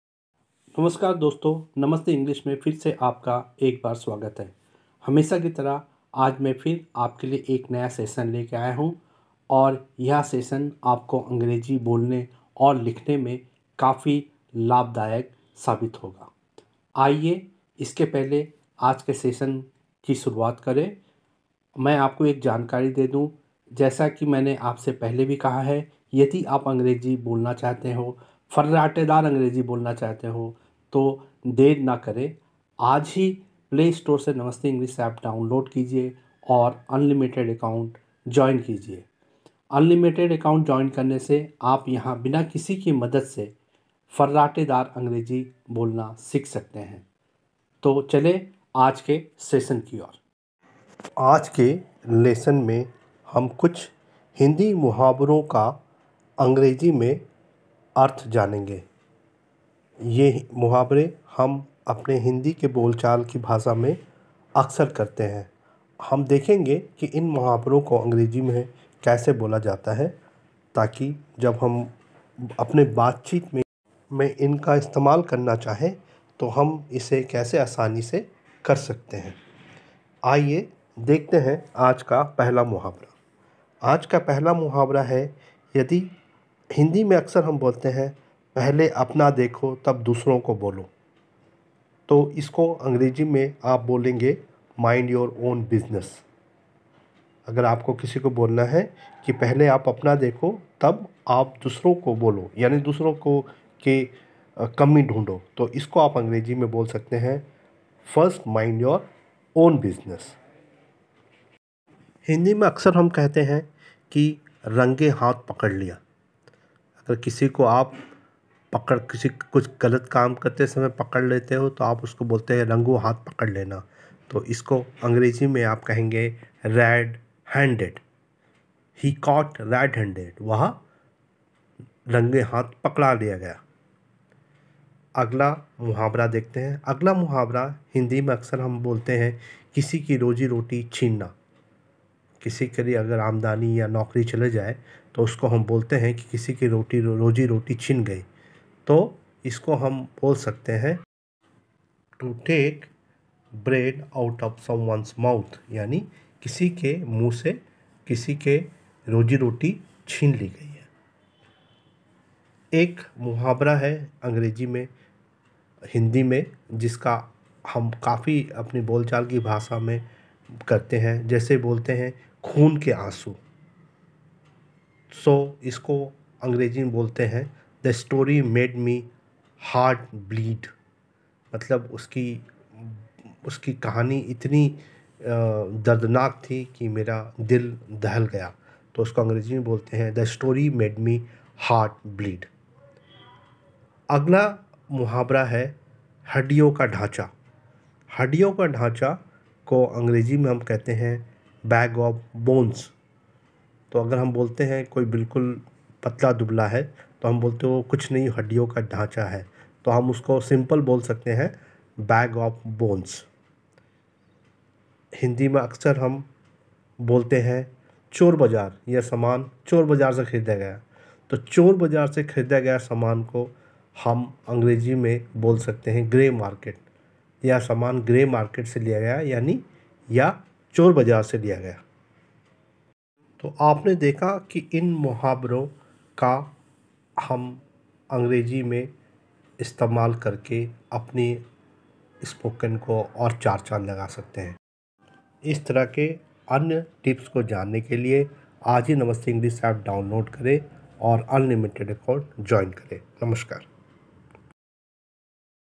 Learn to speak effective English - हिंदी मुहावरों को अंग्रेज़ी में जानें
हिंदी मुहावरों को अंग्रेज़ी में जानें :
पहले अपना देखो तब दूसरों को बोलो → Mind your own business
रँगे हाथों → Red handed
किसी की रोजी रोटी छीनना → To take bread out of somebody’s mouth
खून के आँसू → The story made me heartbleed
हड्डियों का ढांचा → Bag of bones
चोर बाजार → Grey market
काम की बात करना → Talk shop
भगवान की दया पर → At the mercy of God